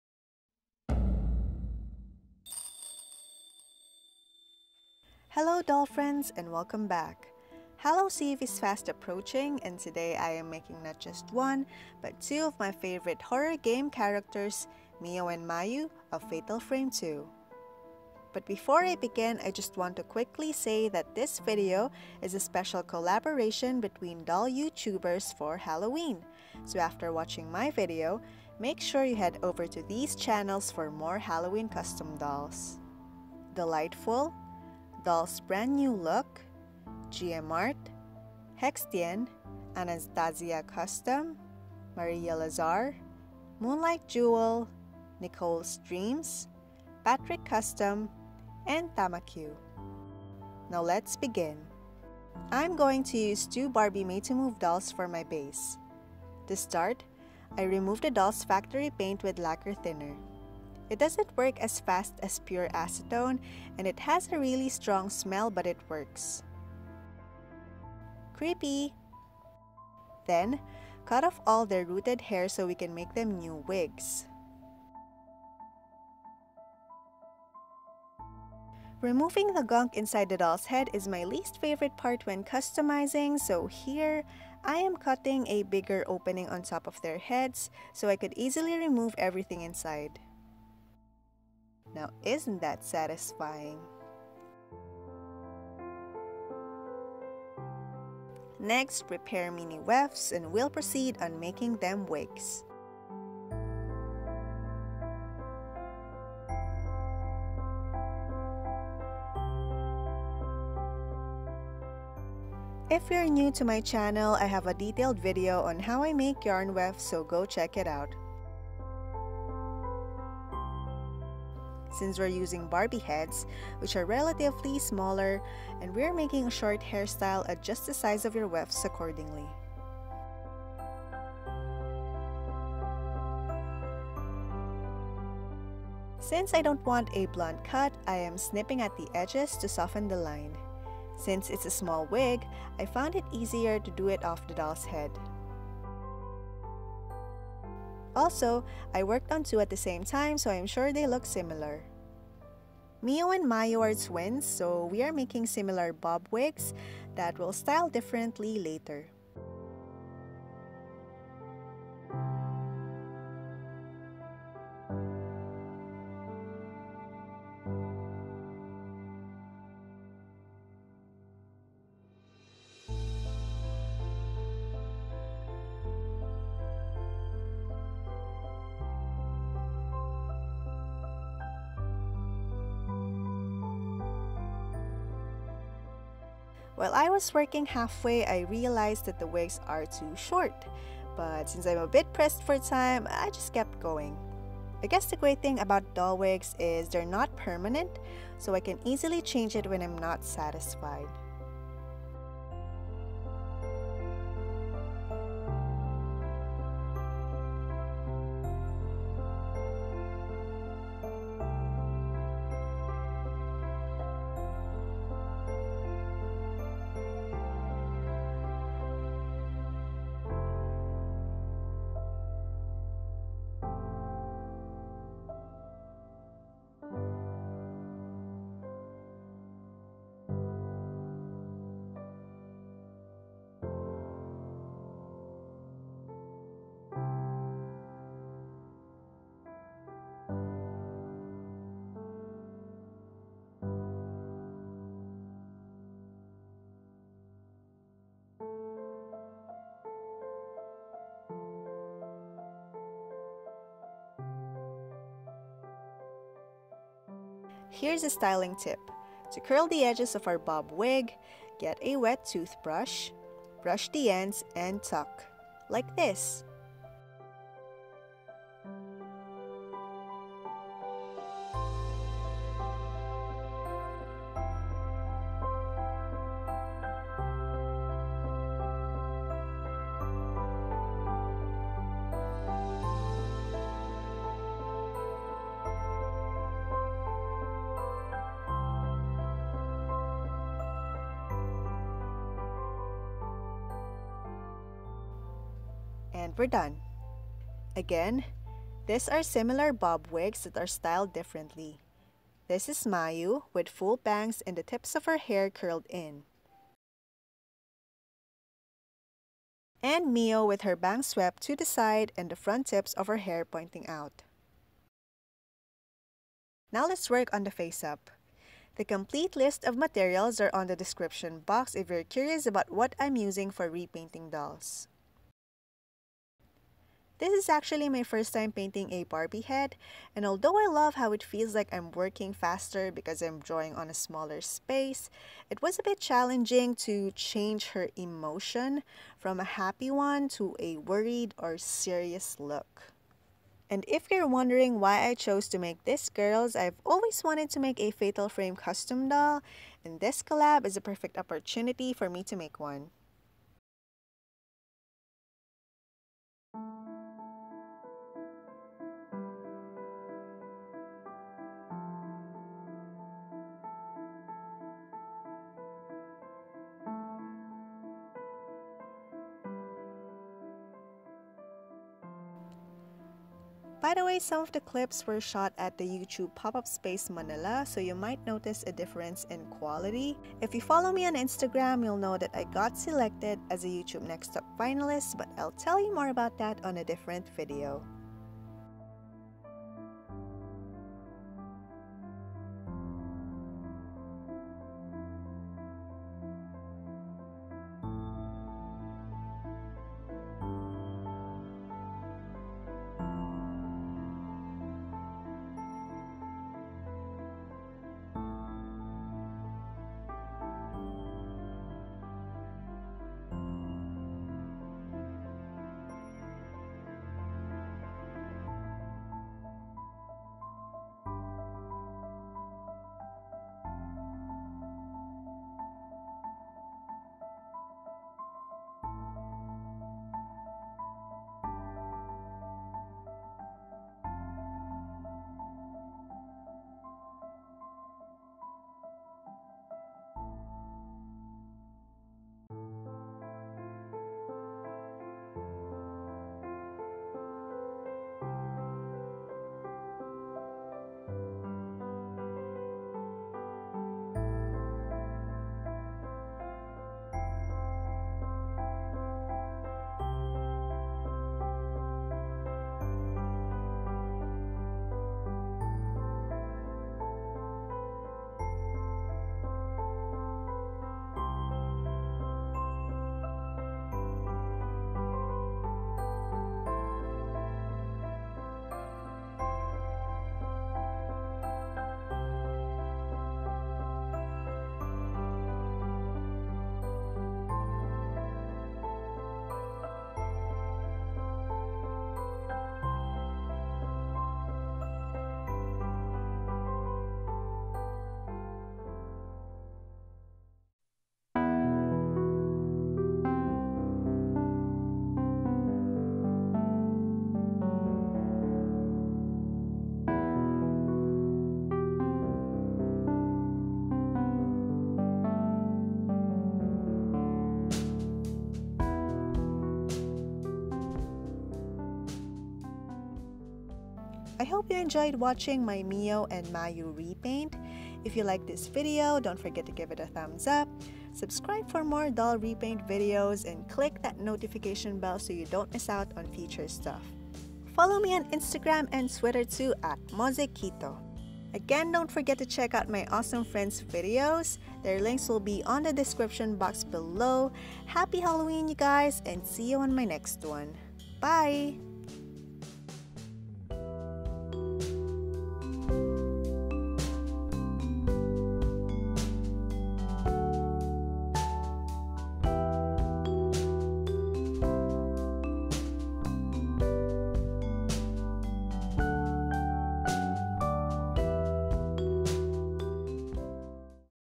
Mio and Mayu (Fatal Frame 2) | Halloween Collaboration | Custom Barbie Doll Repaint | Mozekyto #6
Happy Halloween loves!!🎃👻

Materials used in the video:

Barbie Made to Move
Familia Yarn (dark brown)
Laquer thinner
Vallejo Acryllic Matte Varnish (62.062)
Mr. Super Clear
Prismacolor Watercolor Pencils
Derwent Inktense Pencils
Jack Richenson Handmade Pastels
Acrylique Acrylic Paint (Teal)
Sculpey Gloss Glaze
MaxBond Glue

My videos are now open for subtitle contributions! I'd be really thankful for your help!